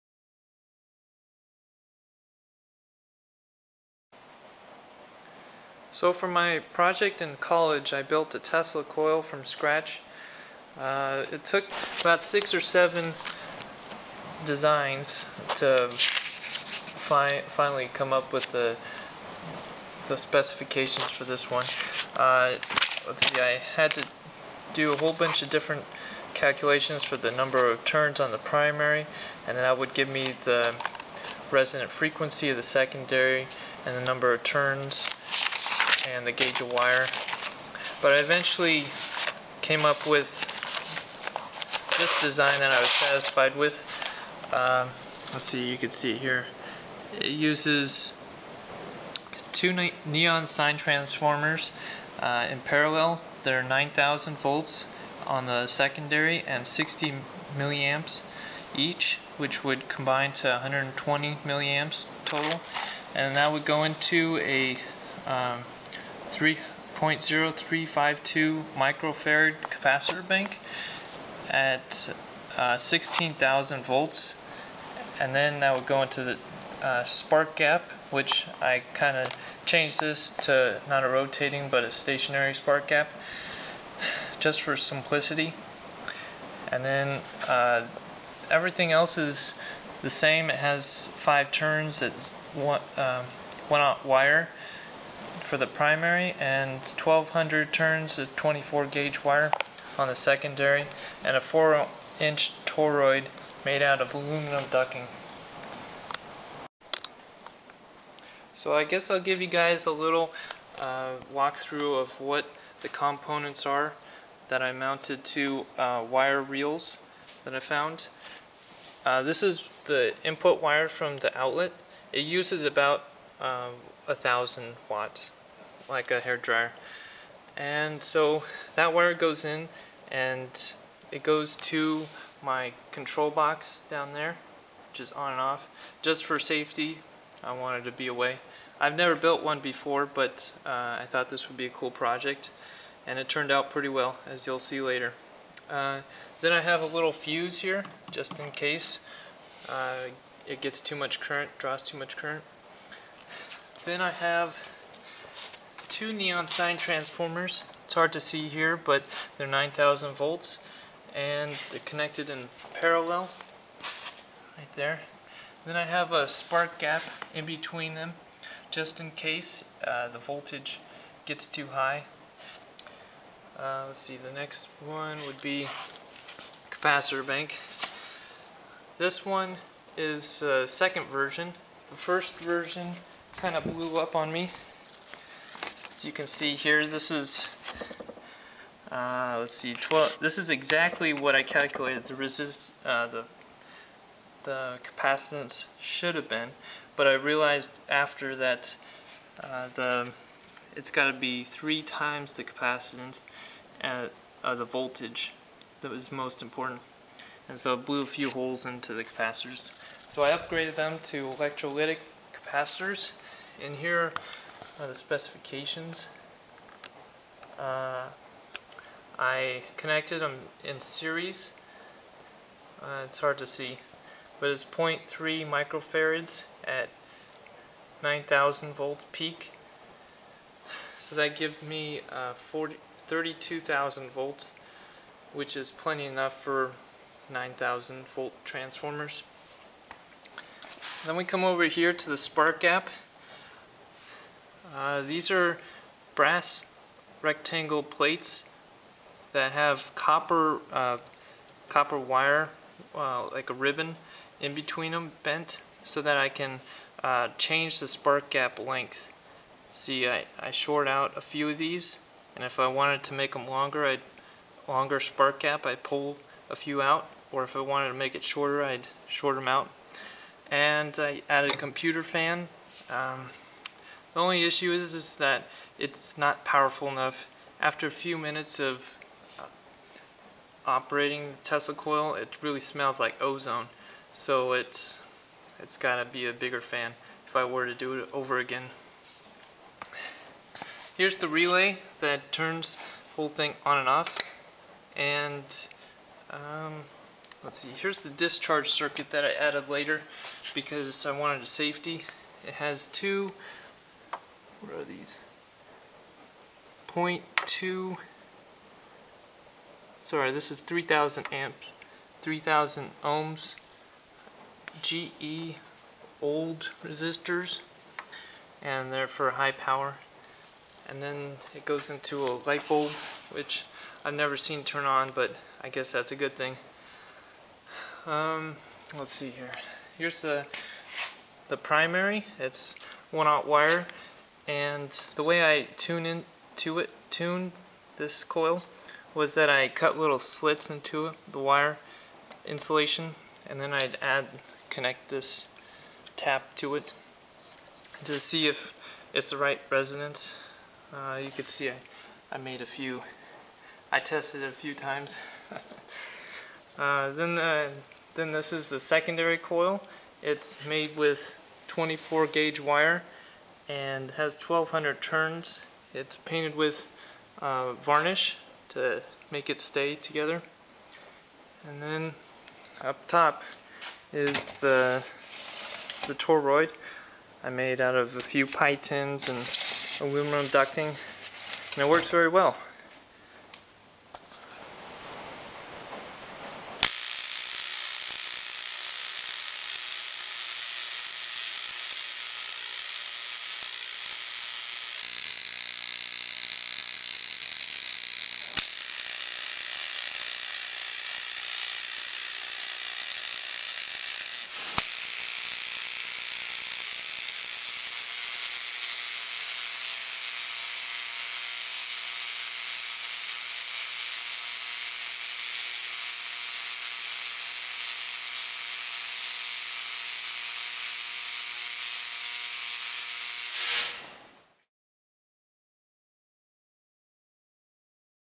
1000 Watt Tesla Coil
This is a video of a homemade 1000 watt Tesla Coil and its operation.